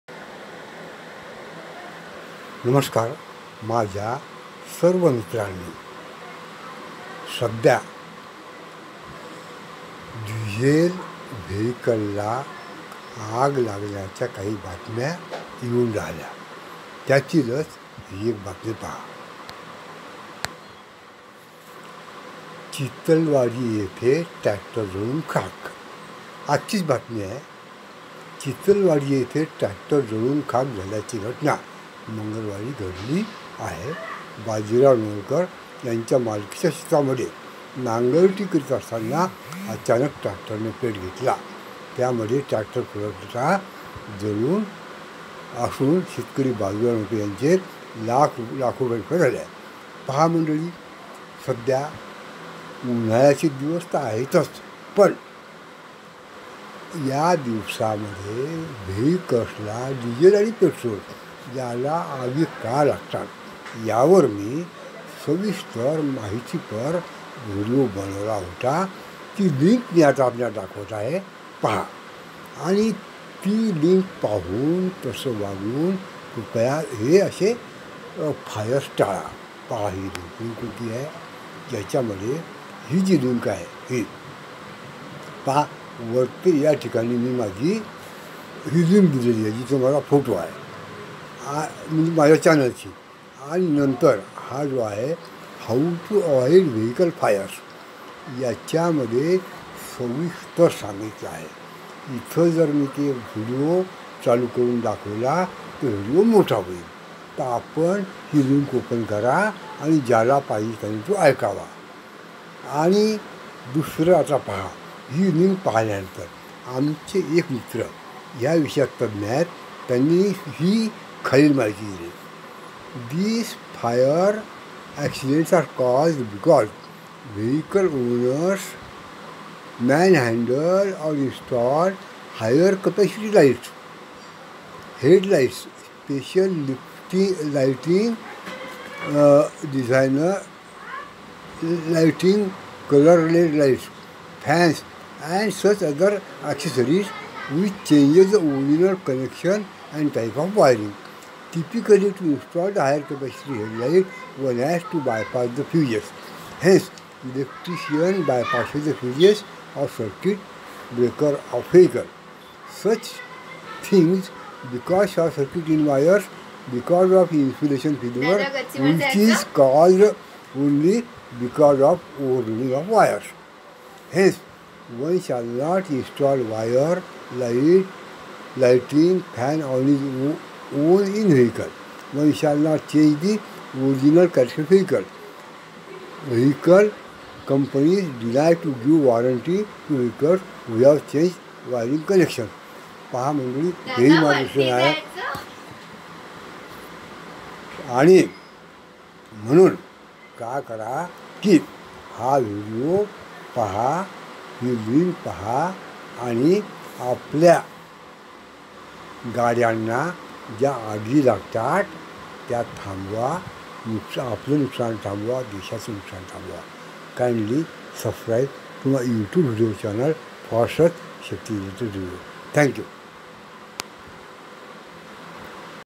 नागाटी करत असताना, अचानक आग लागली व Tractor जलून लाखो रुपयांच नुकसान !
To avoid such types of vehicle fires kindly watch this video please